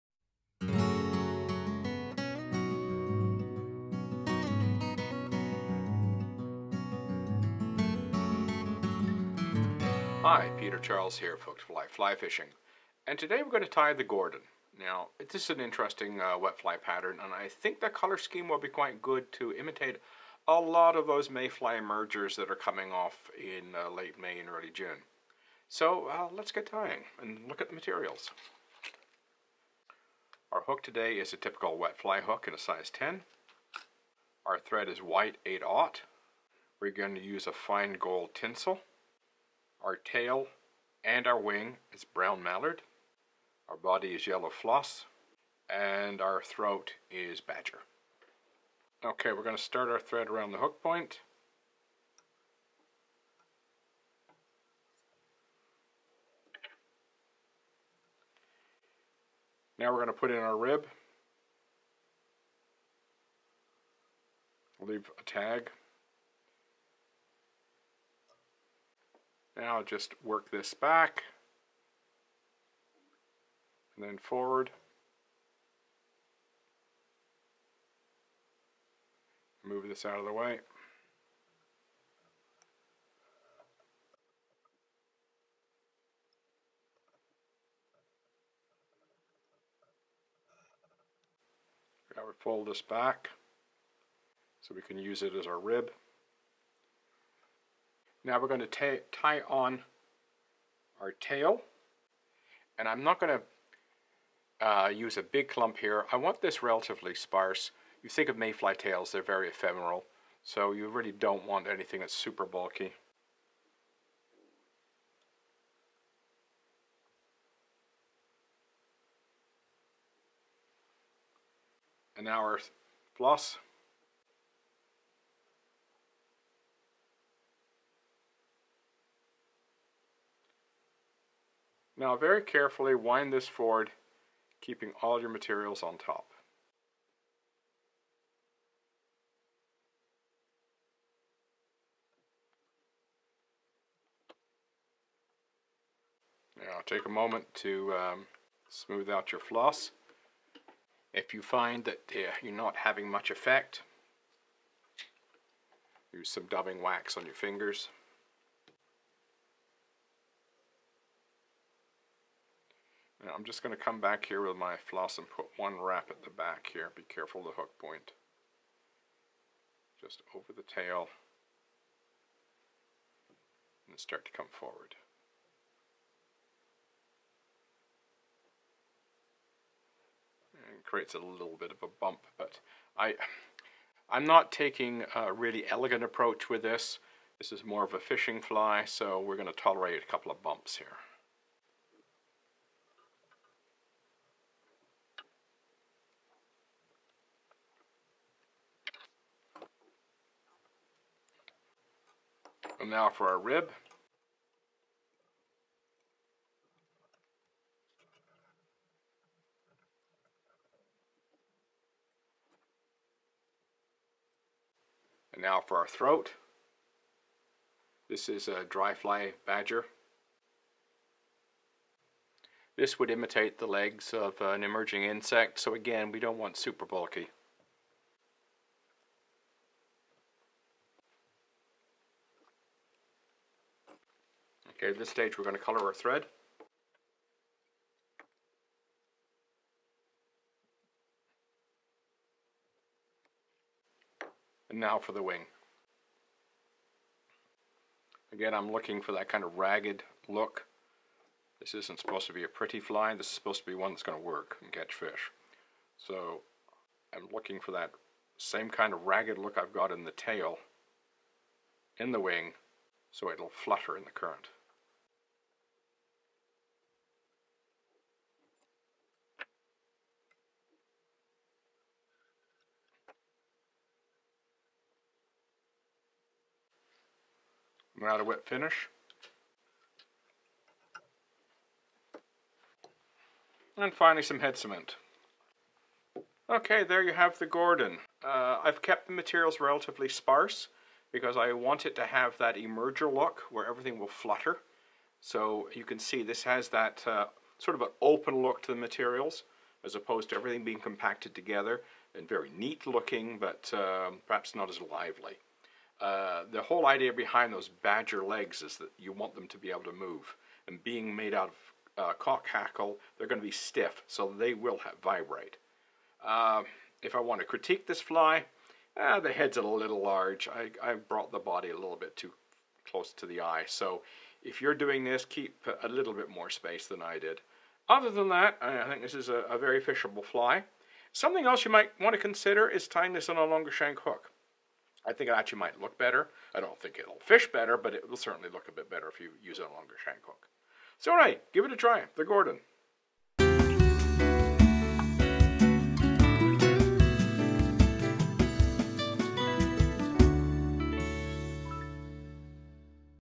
Traditional Wet Fly Trout Patterns: the Gordon
The Gordon should be a great wet fly to fish for trout rising to emergers in the late May early June when so many of the flies hatching have light to yellow bodies.  Give it a try.